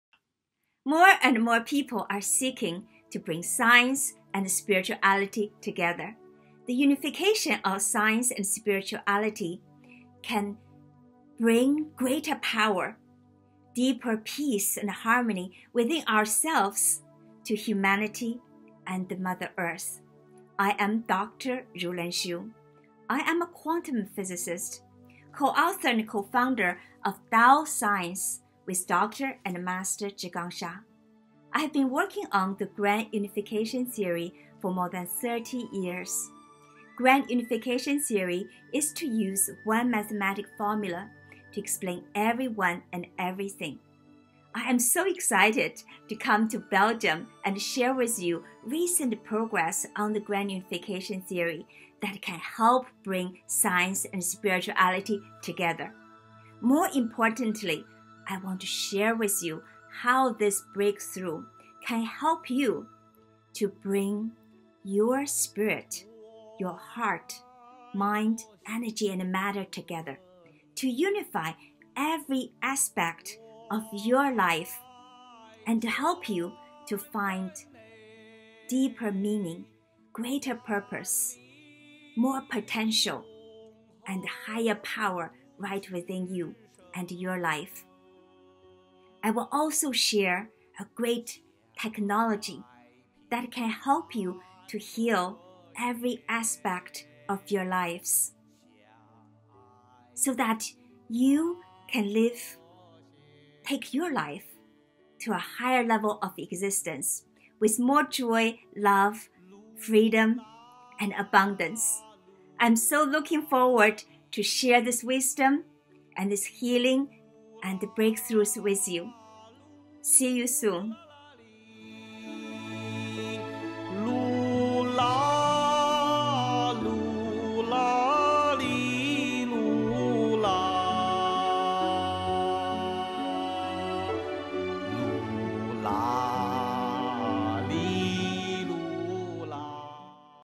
Tao Science Unification of Science & Spirituality Belgium Okt 22 27
Many of us have been searching for answers. Many have been searching for how we could be part of something deeper – something that connects and unites us with each other. Even more so, how we can use this wisdom to transform our lives.

Tao Science is the science of the Source, creation and grand unification of everyone and everything. It unifies science, spirituality, everyone, everything and every aspect of life at a most fundamental level.